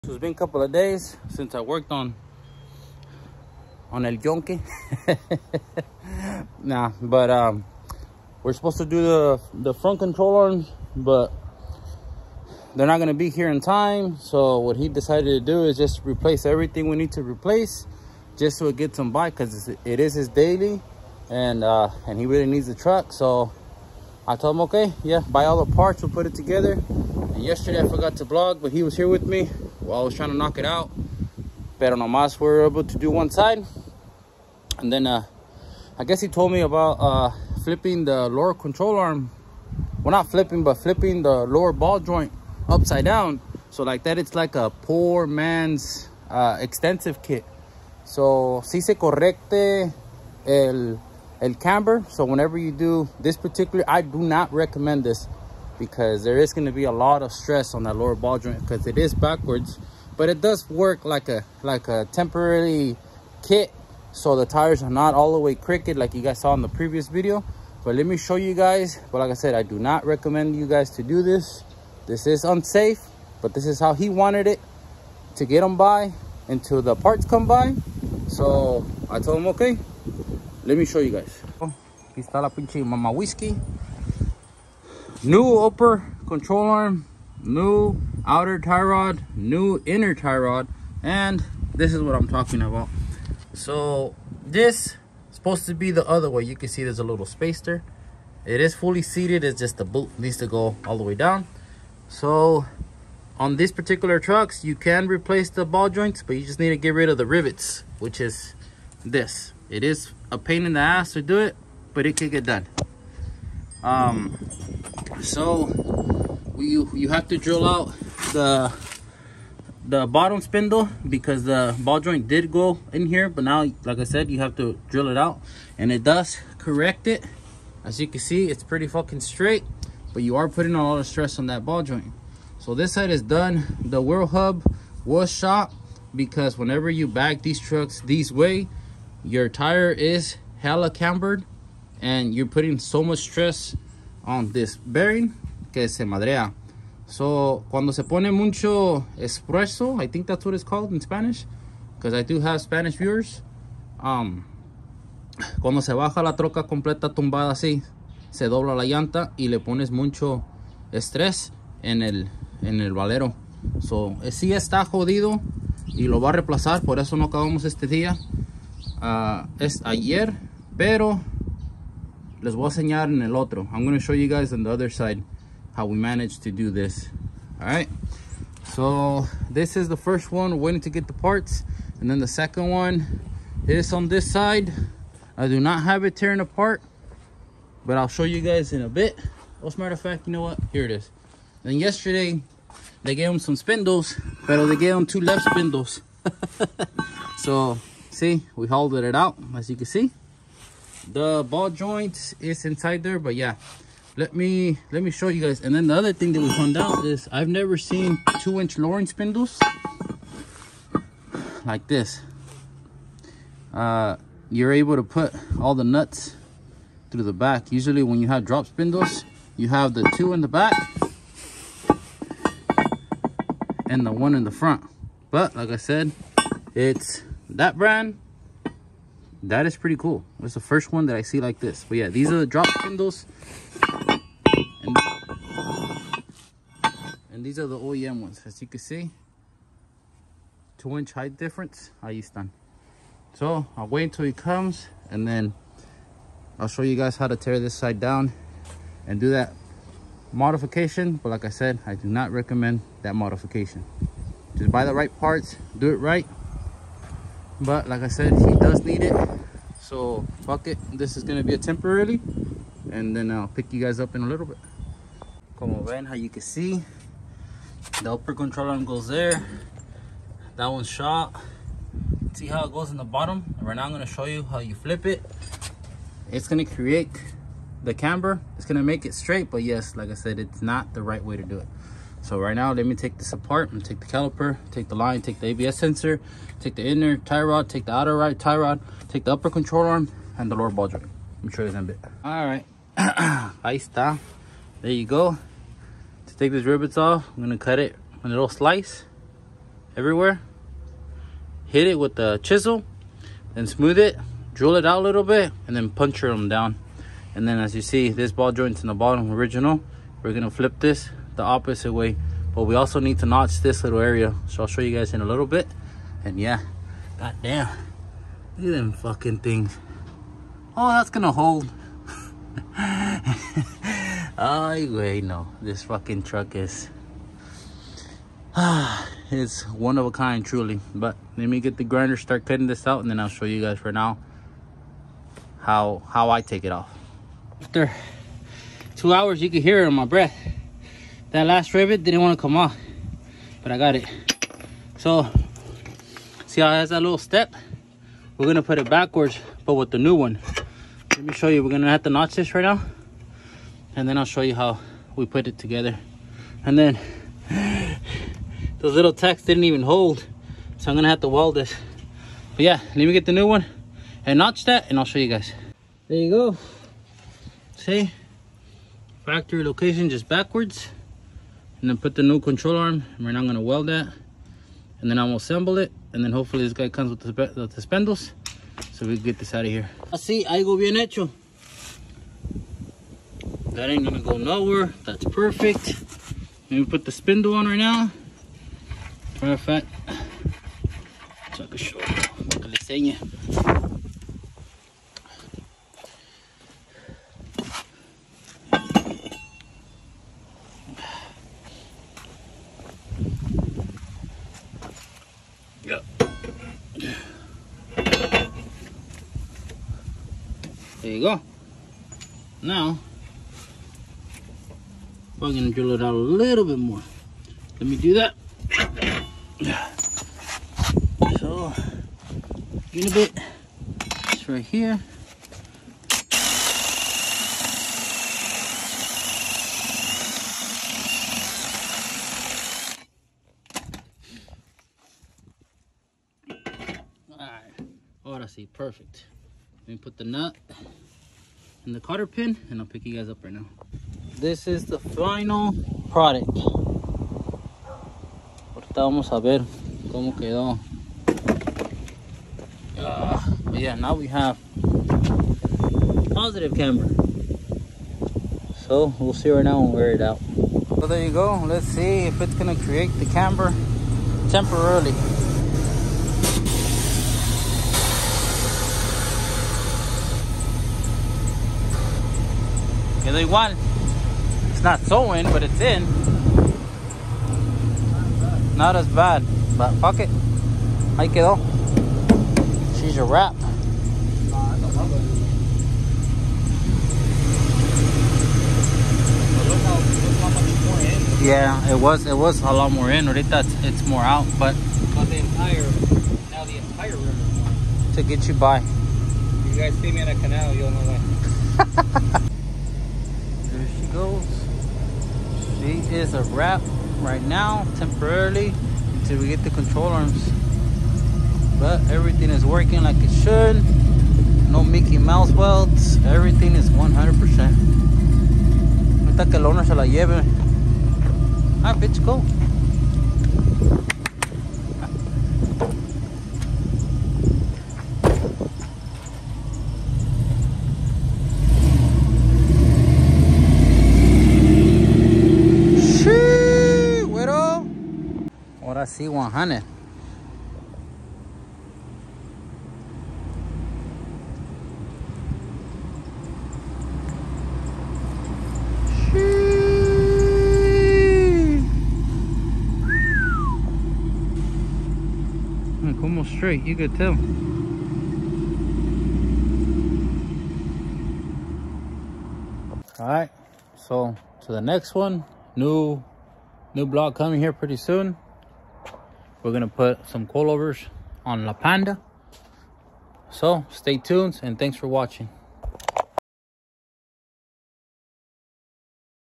2003 Silverado part 2 technical difficulties
03 Silverado technical difficulties control arms are not coming in until two months. We had to get it up and going because my boy needed it as a daily.